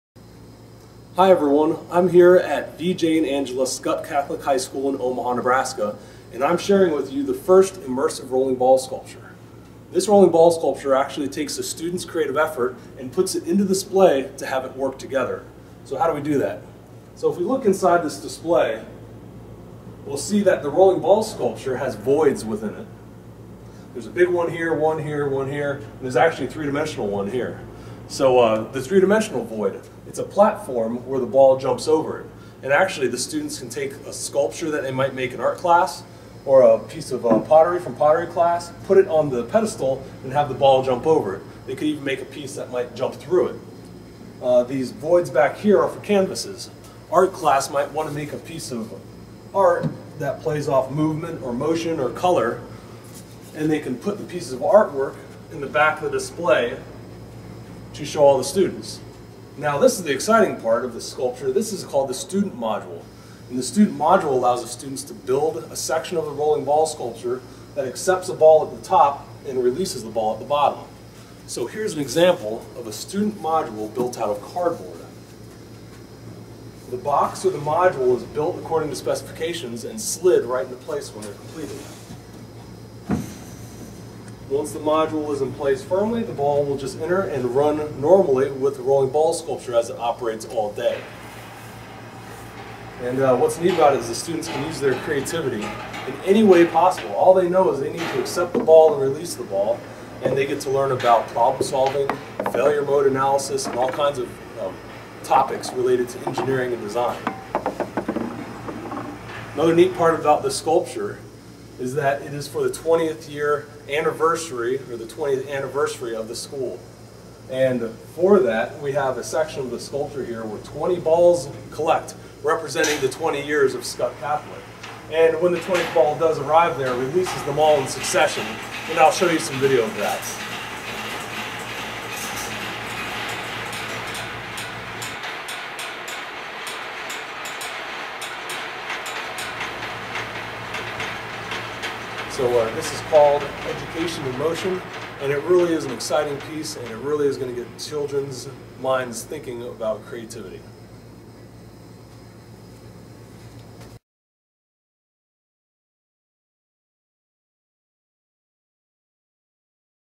"Education in Motion" an Immersive Rolling Ball Sculpture
"Education in Motion," a rolling ball sculpture at Skutt Catholic High School in Omaha, Nebraska, designed and fabricated by Eddie Boes.  This is the first Immersive rolling ball sculpture.  An immersive rolling ball sculpture incorporates the students creative efforts into the rolling ball sculpture through artwork display points and the student module.

The student module allows the students to design and fabricate a section of the rolling ball sculpture.   Each term, a new group of students can design a new student module and watch it run continuously with the rest of the sculpture.   This is a great way to get students interested in STEM subjects and creativity.

-Eddie